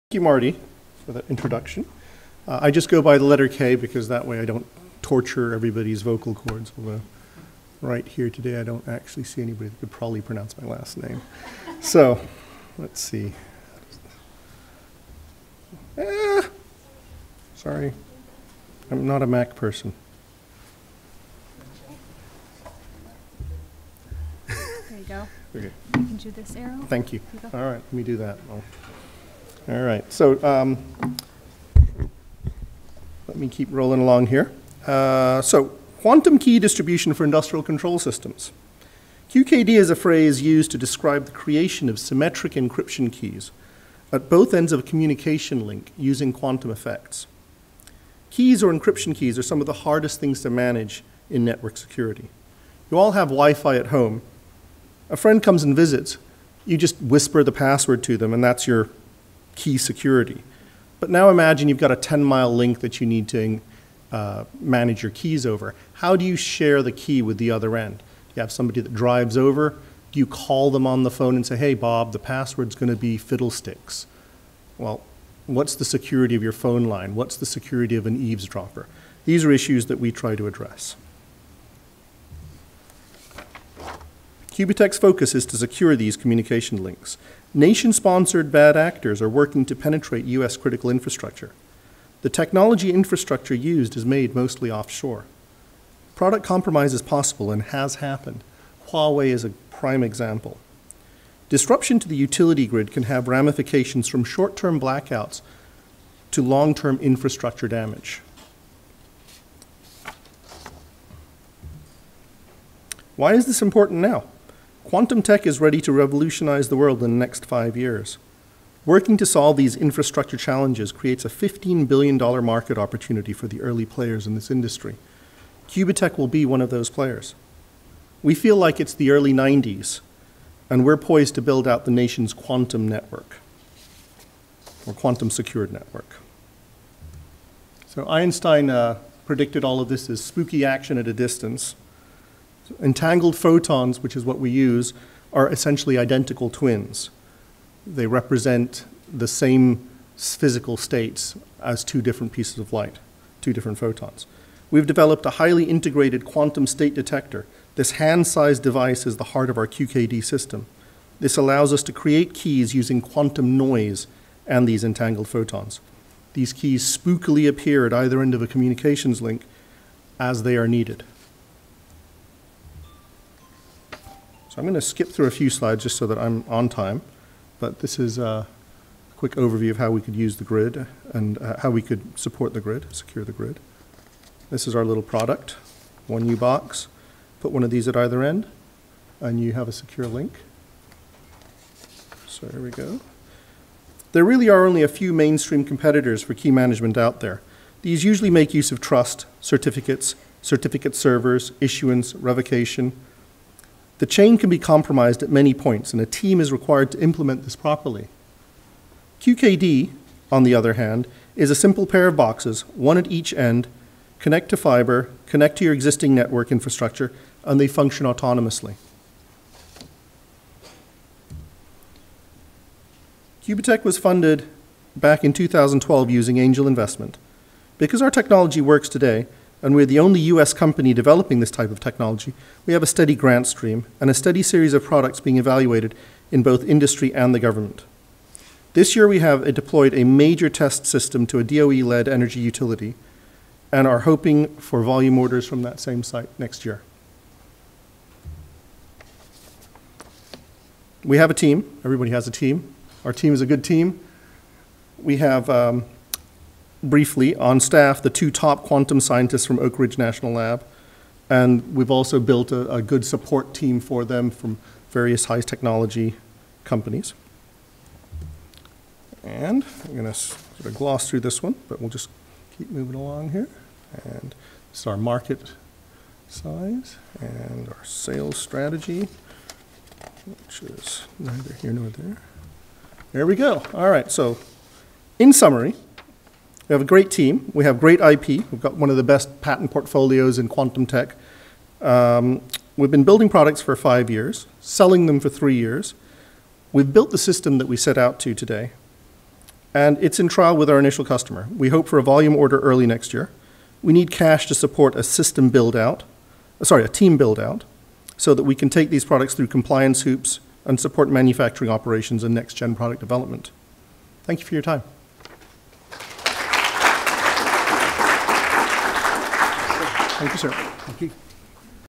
Qubitekk Presents at 2019 Southern California Energy Innovation Network Showcase
“K” Karunaratne, VP of Engineering, Qubitekk, presents at a Cleantech San Diego event titled Southern California Energy Innovators Transforming the Energy Scene (September 10, 2019)